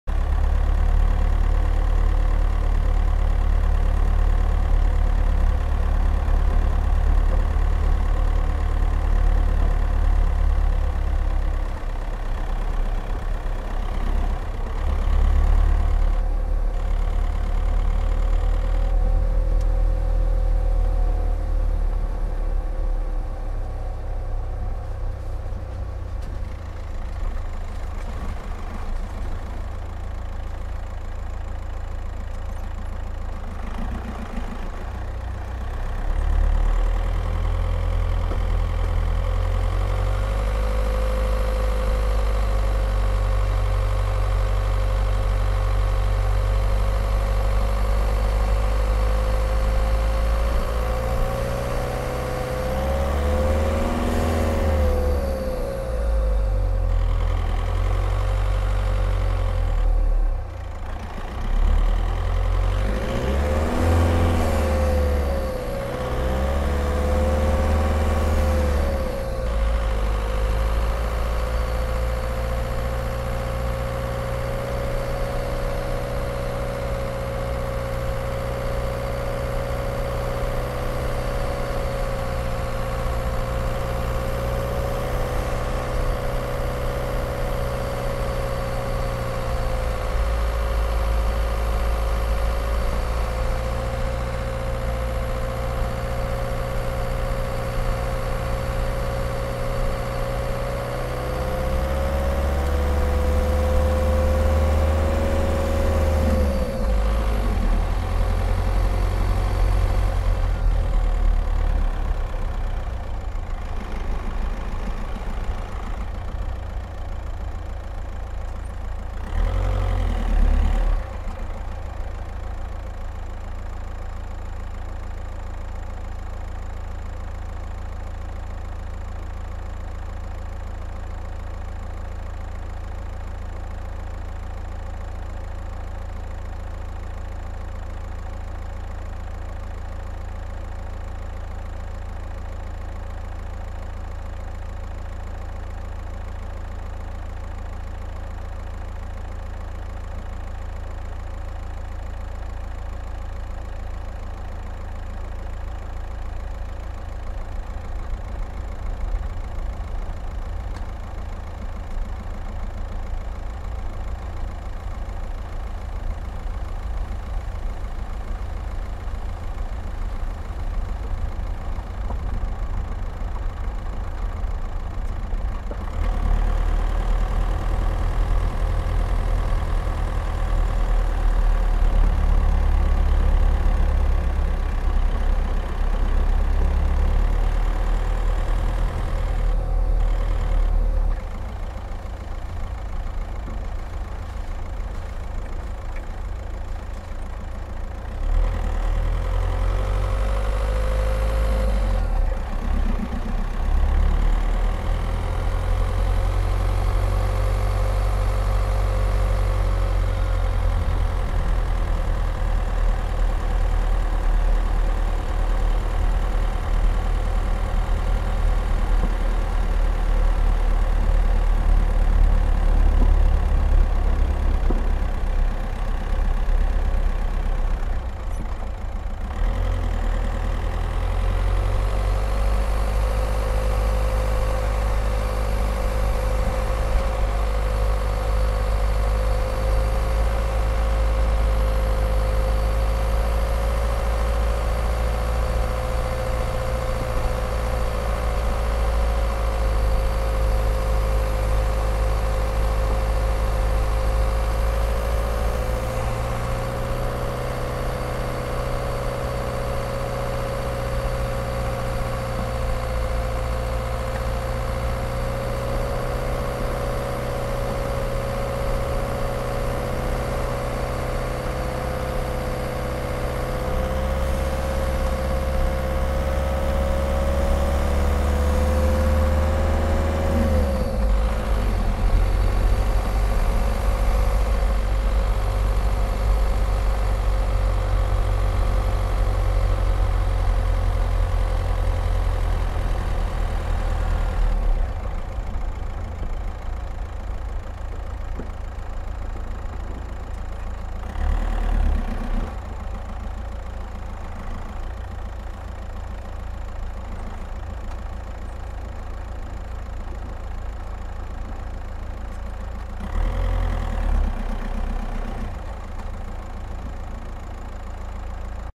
Skoda Octavia 1.9 TDI - Winter offroad test.
Skoda Octavia goes  offroad to Dambovicioara in winter conditions, on a mud and snow road. Winter tires Nokian WR D4.
Video: Gopro 4 Silver 1080p